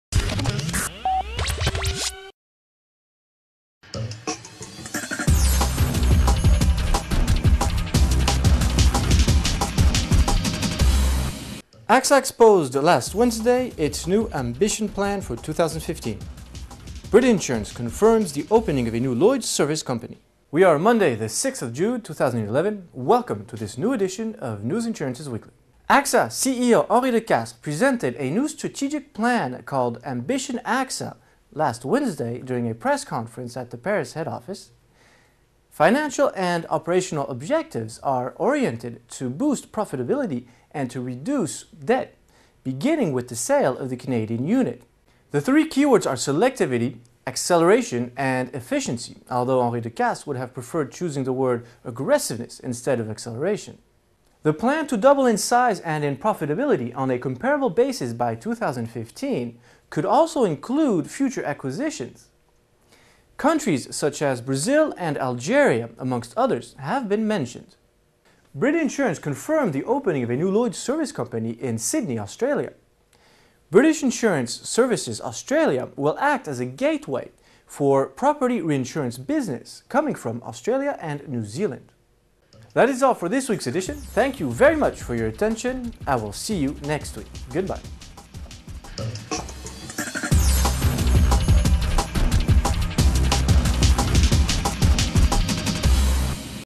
Insurance News Weekly talks about Ambition AXA
At the AXA head office in Paris this week, Henri de Castries, AXA's CEO, presented the new strategic plan called "Ambition AXA".
The financial and operational objectives are oriented to boost profitability and to reduce debt. The ambition is to double in size by 2015. Expansion in high profitable markets may include future acquisitions; Brazil and Algeria are countries amongst others that have been named.
British Insurance has confirmed the opening of a Lloyd's service company in Sydney Australia. The new unit will act as a gateway for all property treaty reinsurance businesses coming from Australia and New Zealand.